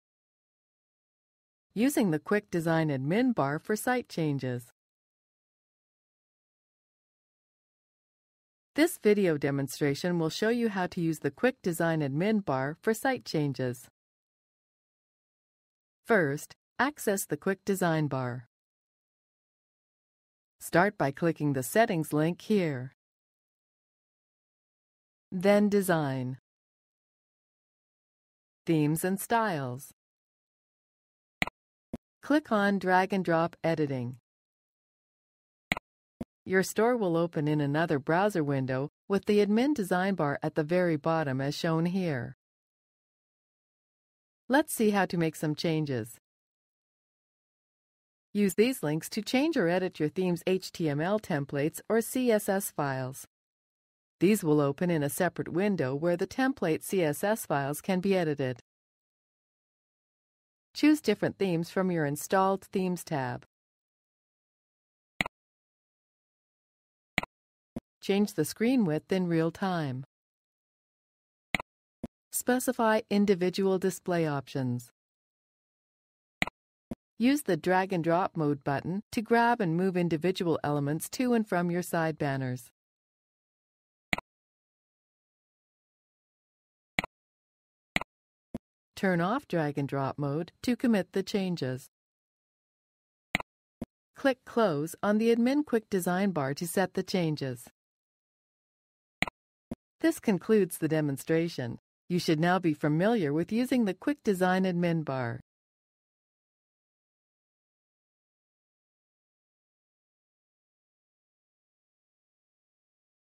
Using the Quick Design Bar - 3dCart Shopping Cart Software
How to use 3dCart's Quick Design Bar for Site Changes. To learn more about the 3dCart e-Commerce Solution, please